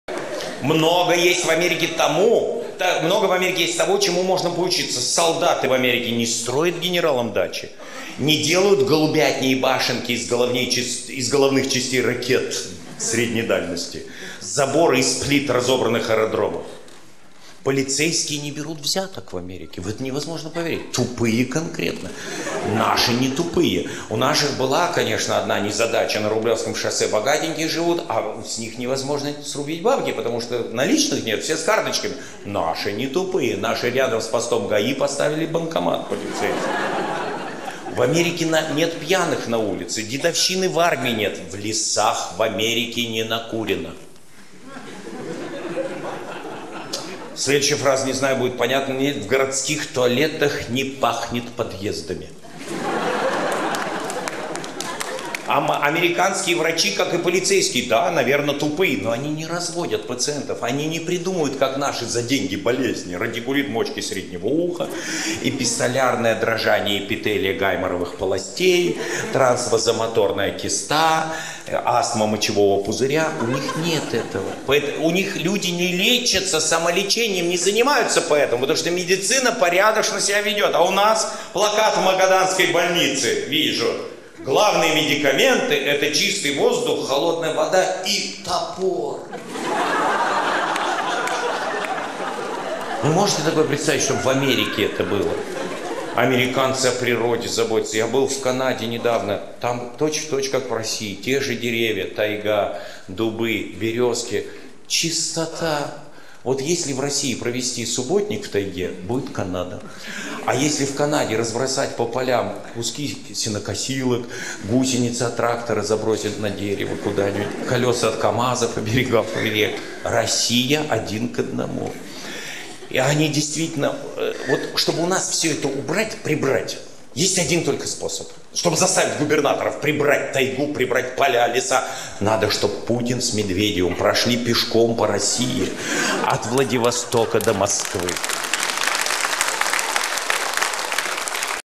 Михаил Задорнов. Чему нам учиться у Америки?
Американские солдаты не строят генералам дачи, на улицах нет пьяных и даже в городских туалетах не пахнет подъездами! Американские врачи не разводят пациентов.